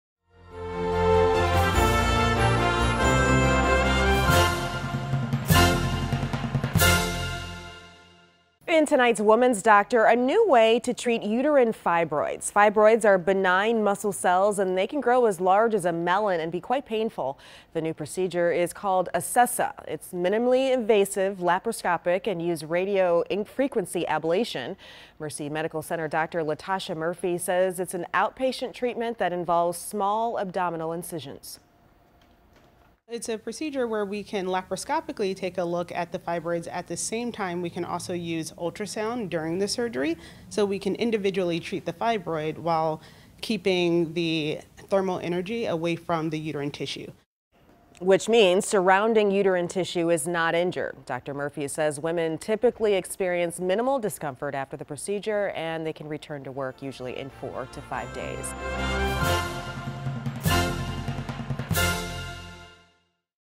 New Treatment for Uterine Fibroids - Dr. Latasha Murphy - Mercy
Gynecologist Dr. Latasha Murphy discusses Acessa, which is a new treatment for uterine fibroids.